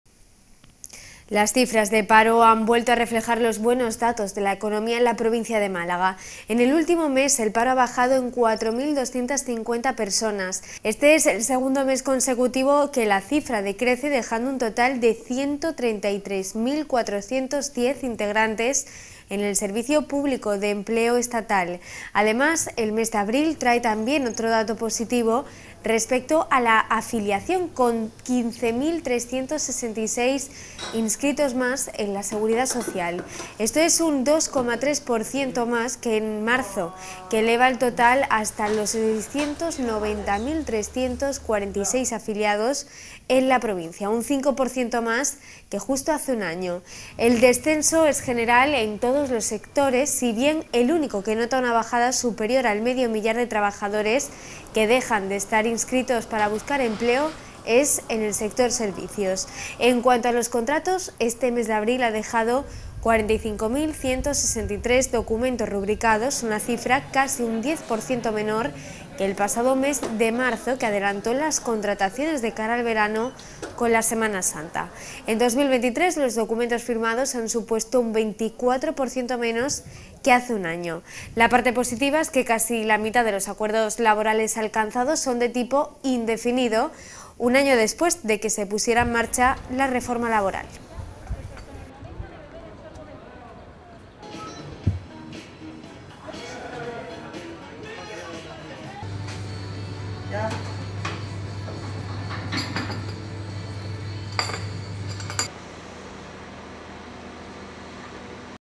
🔴Noticia - El paro baja en Málaga en 4.250 personas
Las cifras de paro vuelven a reflejar los buenos datos de la economia en la provincia de Málaga. En el último mes, el paro ha bajado en 4.250 inscritos. Este es el segundo mes consecutivo que la cifra decrece, dejando un total de 133.410 integrantes en el Servicio Público de Empleo Estatal Además, el mes de abril trae también otro dato positivo respecto a la afiliación, con 15.366 inscritos más en la Seguridad Social. El descenso es general en todos los sectores, si bien el único que nota una bajada superior al medio millar de trabajadores que dejan de estar inscritos para buscar empleo es el sector servicios. En cuanto a los contratos, este mes de abril ha dejado 45.163 documentos rubricados, una cifra casi un 10% menor que el pasado mes de marzo, que adelantó las contrataciones de cara al verano con la Semana Santa. en 2023 los documentos firmados han supuesto un 24% menos que hace un año. La parte positiva es que casi la mitad de los acuerdos laborales alcanzados son de tipo indefinido, un año después de que se pusiera en marcha la reforma laboral.

English:

The unemployment figures once again reflect the good economic data in the province of Malaga. In the last month, unemployment has dropped by 4,250 registered. This is the second consecutive month that the figure has decreased, leaving a total of 133,410 members in the State Public Employment Service. This is 2.3% more than in March, which brings the total to 690,346 members in the province, 5% more than a year ago. The decrease is general in all sectors, although the only one that notes a decrease of more than five thousand workers who are no longer registered to seek employment is the services sector. As for contracts, this month of April it has left 45,163 initialed documents, a figure almost 10% lower than last March, which brought forward hiring for the summer with Easter. in 2023 the documents signed have represented 24% less than a year ago. The positive part is that almost half of the labor agreements reached are of an indefinite type, one year after the labor reform was launched.

Live streaming